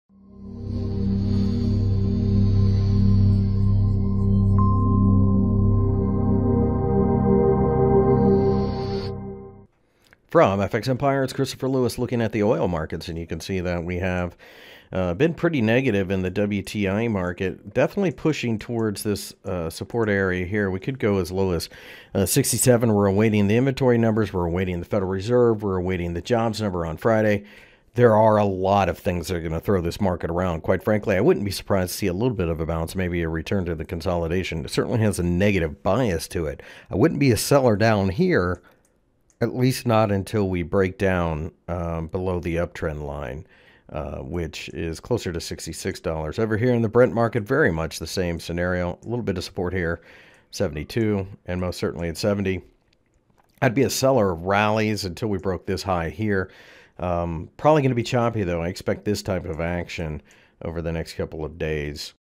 Oil Technical Analysis for August 02, 2018 by FXEmpire.com
As crude oil markets awaited inventory numbers for the week, they fell significantly yet again. For the full article go - Crude Oil Forecast August 02, 2018, Technical Analysis.Find more information about Forex and Commodities News Technical Analysis Fundamental Analysis and Brokers on FX Empire